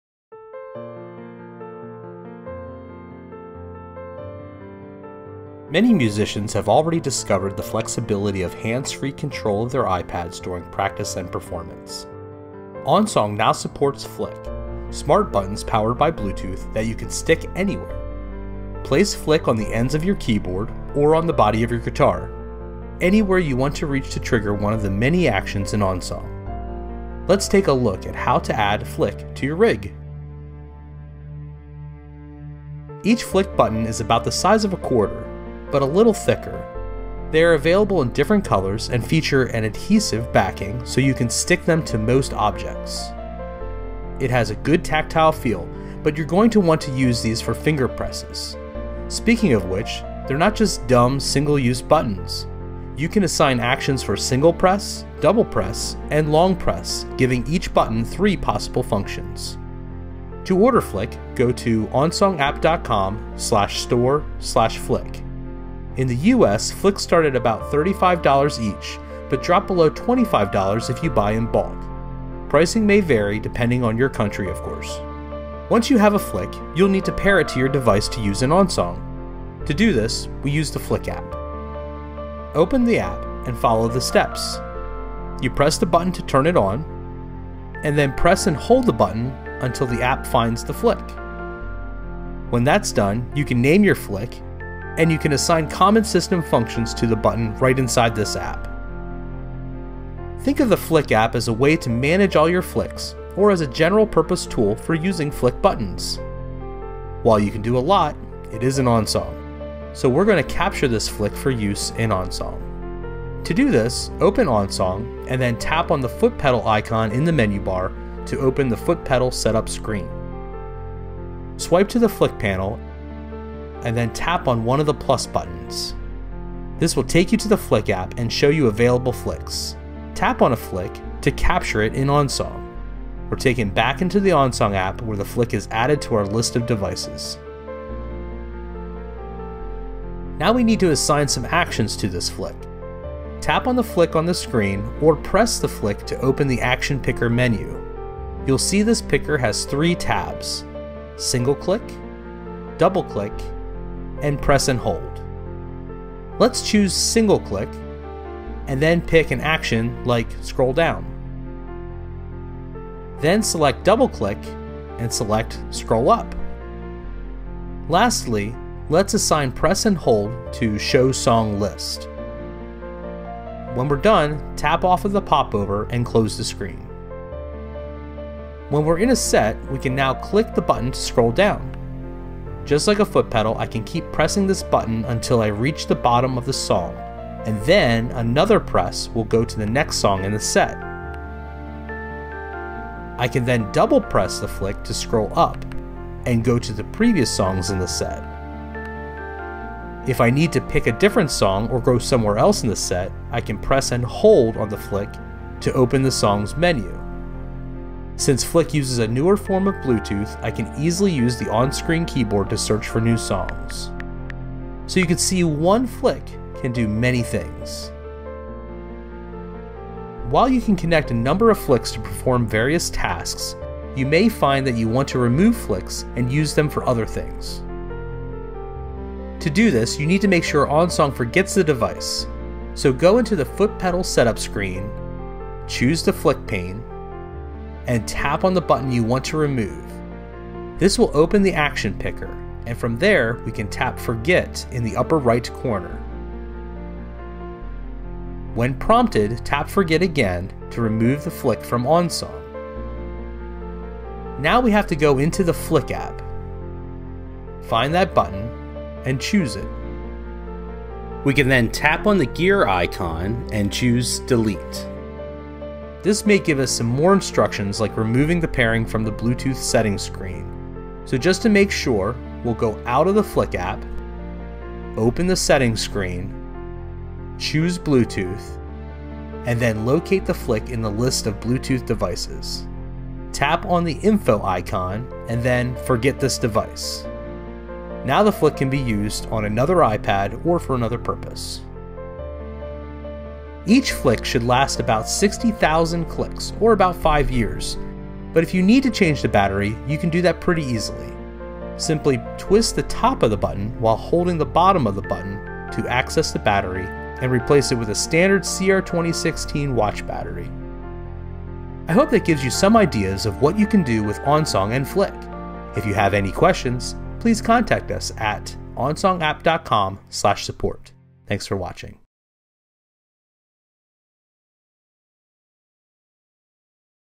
Perform Actions with Flic Smart Buttons
Many musicians have already discovered the flexibility of hands-free control of their iPads during practice and performance.  OnSong now supports Flic – smart buttons powered by Bluetooth that you can stick anywhere. Place Flic on the ends of your keyboard, or on the body of your guitar... anywhere you want to reach to trigger one of the many actions in OnSong.

In this video tutorial we review Flic smart buttons and how to get them connected to OnSong.  We then show you how to set up a single Flic to navigate through your songs and sets. You can use many Flics to perform all of the functions you need in a live performance.